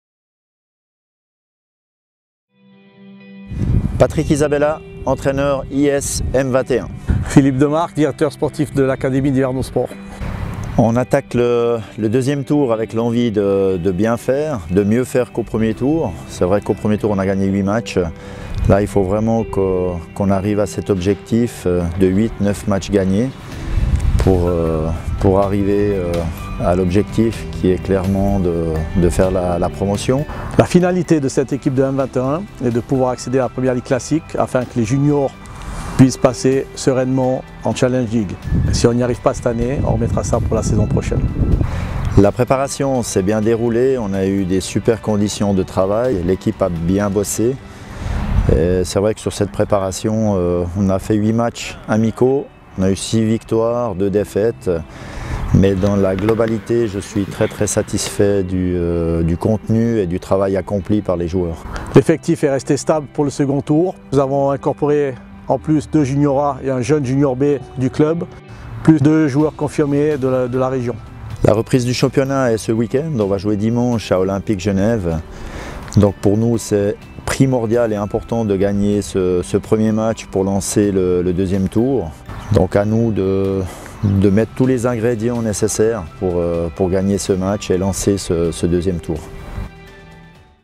Yverdon Sport M21 : reprise, effectif et ambitions
YS M21 reprend le championnat de 2L inter ce week-end. Co-leader du classement, les joueurs de Patrick Isabella veulent se donner les moyens de rester en haut du tableau cette saison, et ainsi aller chercher la 1L classique, objectif recherché par l'équipe réserve à moyen terme.